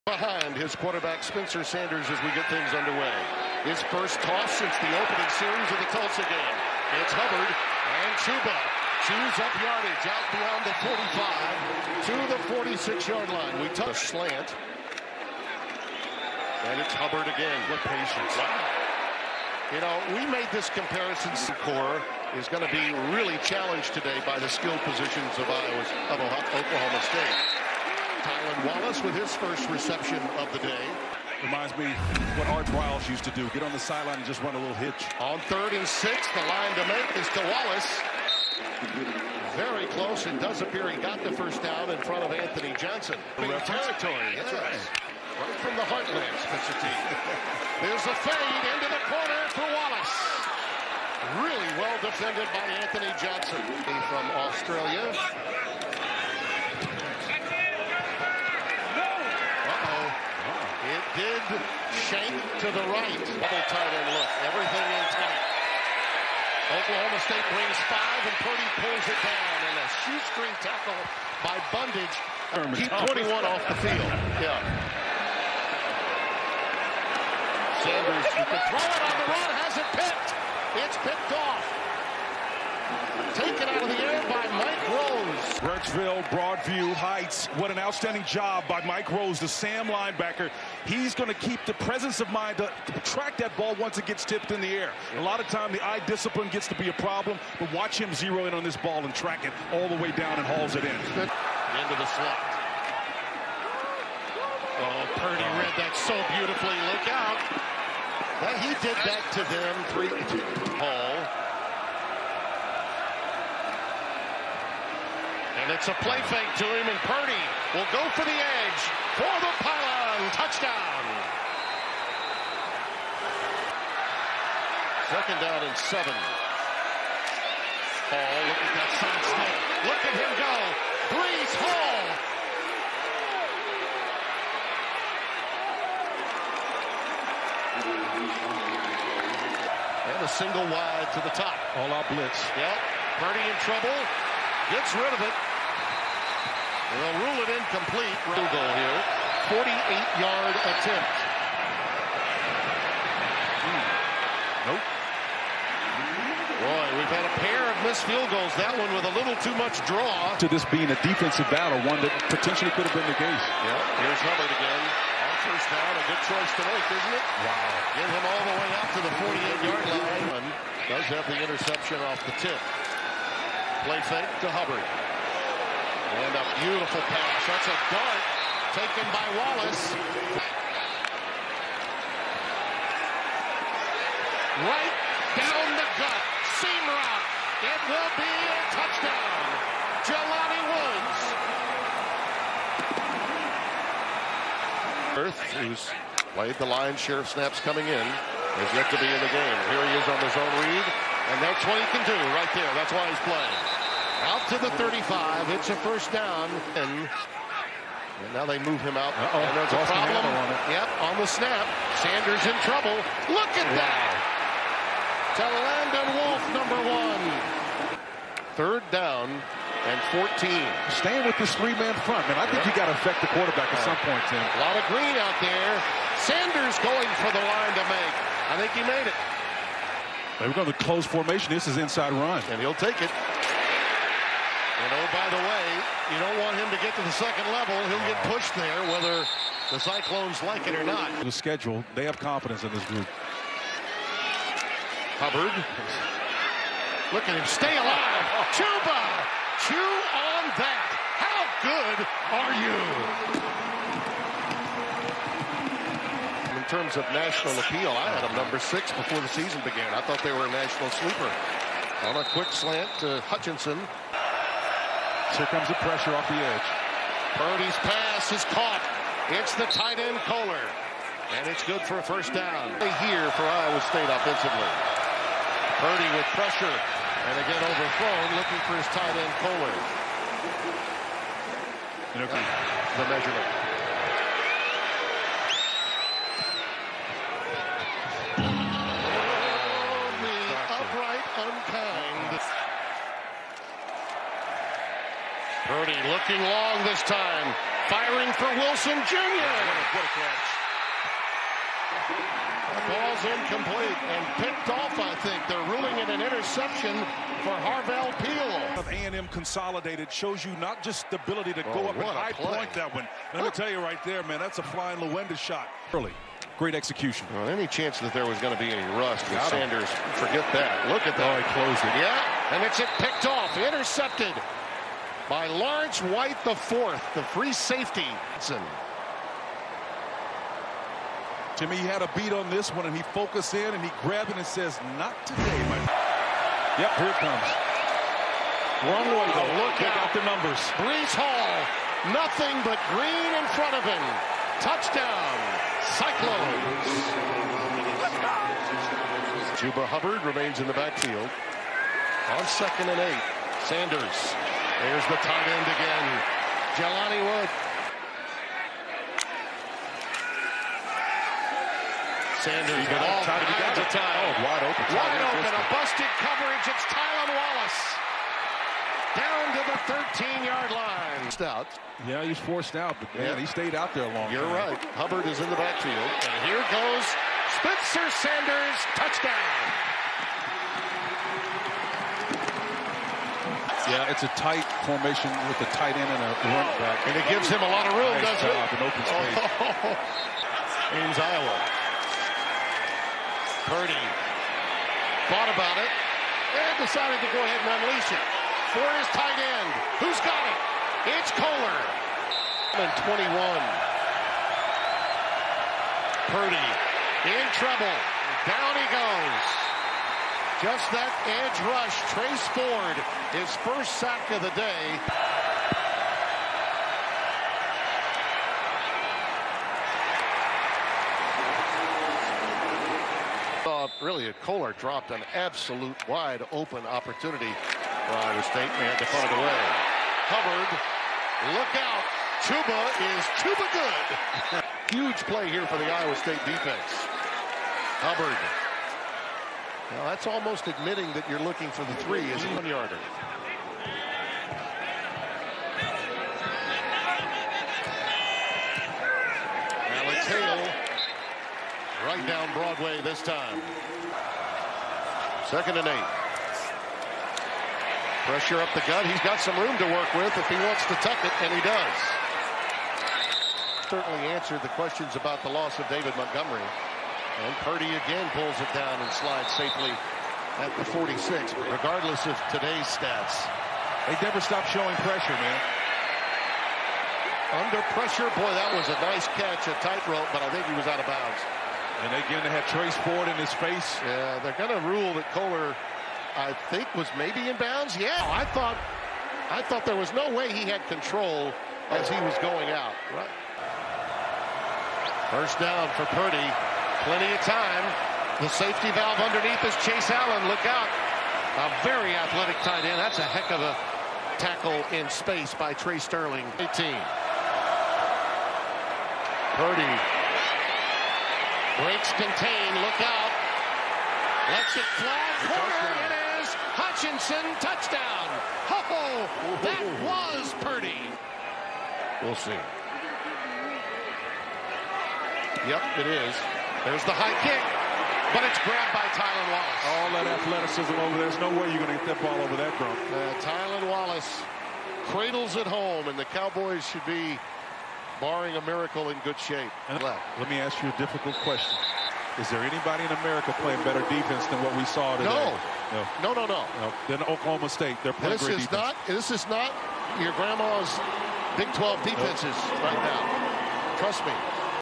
#17 Iowa State vs #6 Oklahoma State Highlights | Week 8 2020 College Football Highlights
Iowa State vs Oklahoma State. Week 8 2020 College Football Highlights of Iowa State vs Oklahoma State.  on 10/24/2020